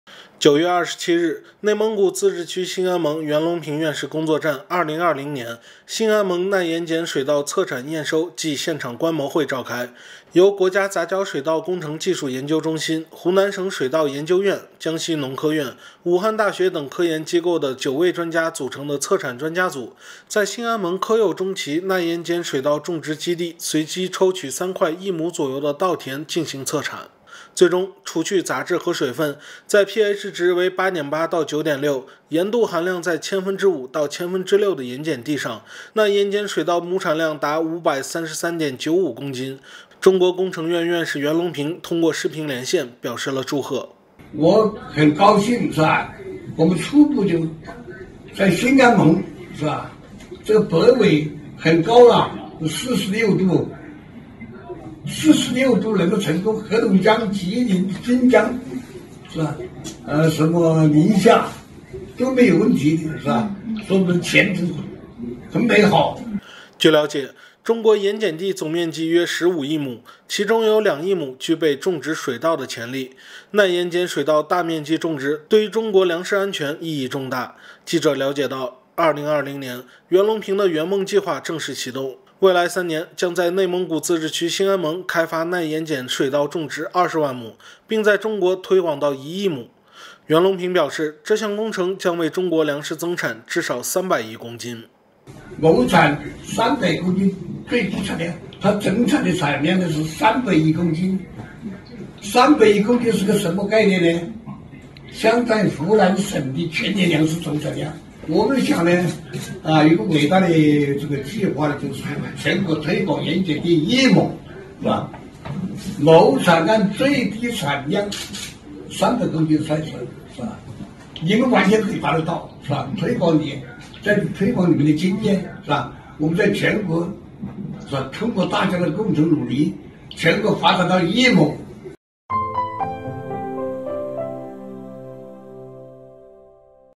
内蒙古兴安盟耐盐碱水稻亩产逾533公斤 袁隆平：可全国推广
9月27日，内蒙古自治区兴安盟袁隆平院士工作站2020年•兴安盟耐盐碱水稻测产验收暨现场观摩会召开。由国家杂交水稻工程技术研究中心、湖南省水稻研究院、江西农科院、武汉大学等科研机构的9位专家组成的测产专家组，在兴安盟科右中旗耐盐碱水稻种植基地随机抽取3块1亩左右的稻田进行测产。最终，除去杂质和水分，在PH值为8.8-9.6、盐度含量在5‰-6‰的盐碱地上，耐盐碱水稻亩产量达533.95公斤。中国工程院院士袁隆平通过视频连线表示了祝贺。

据了解，中国盐碱地总面积约15亿亩，其中有2亿亩具备种植水稻的潜力，耐盐碱水稻大面积种植，对于中国粮食安全意义重大。记者了解到，2020年，袁隆平的“袁梦计划”正式启动，未来3年，将在内蒙古自治区兴安盟开发耐盐碱水稻种植20万亩，并在中国推广到1亿亩。袁隆平表示，这项工程将为中国粮食增产至少300亿公斤。（马知远 张玮）